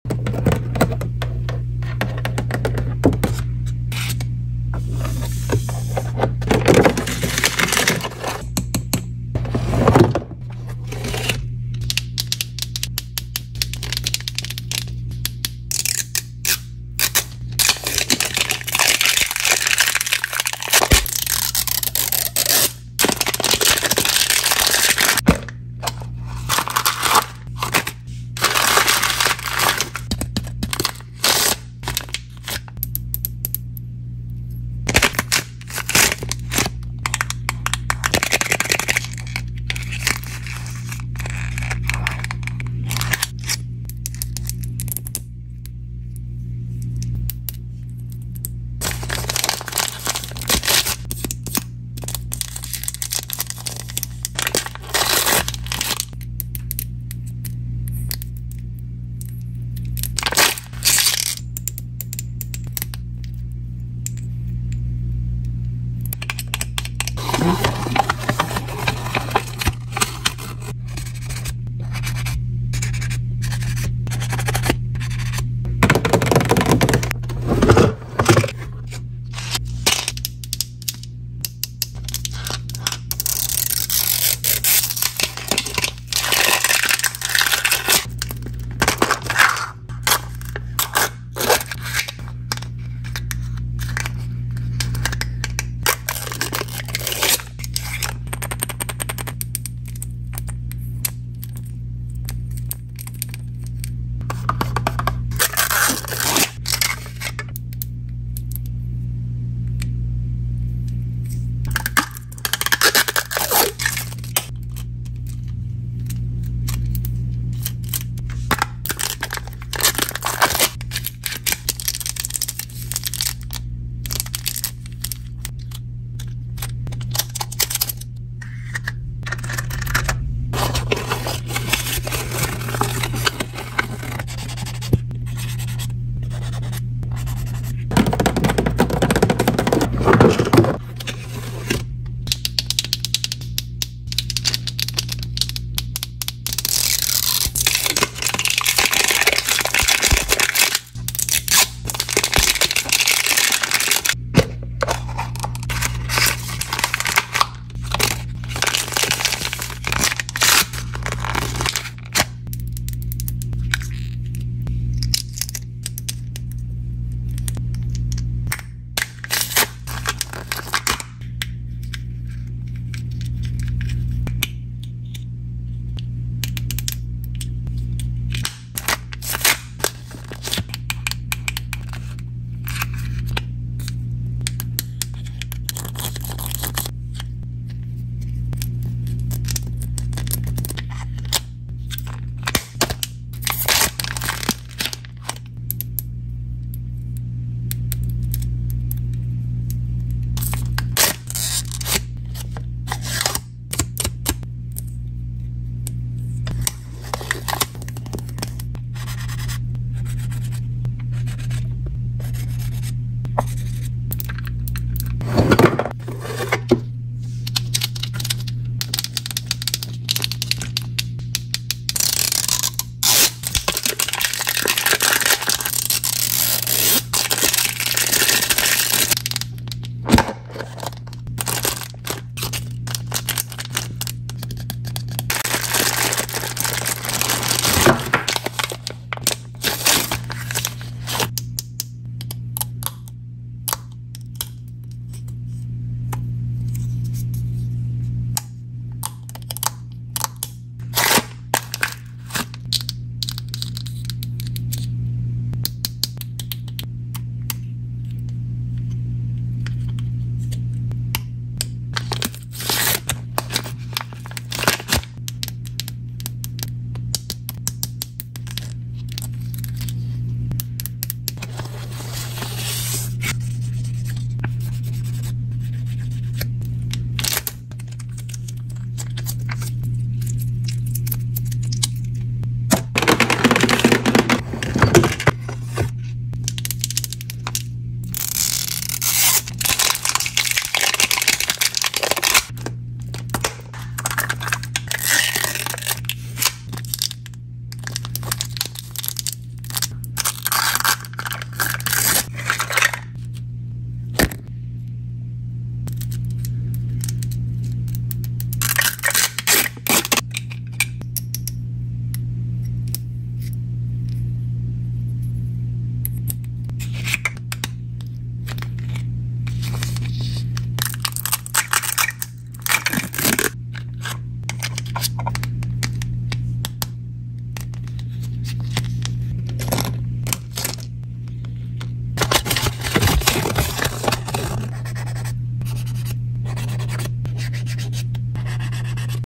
Mini Brands Mini Food Vending Machine Opening Satisfying Video ASMR!
Let's Open this Mini Brands Mini Food Vending Machine to See What's Inside?!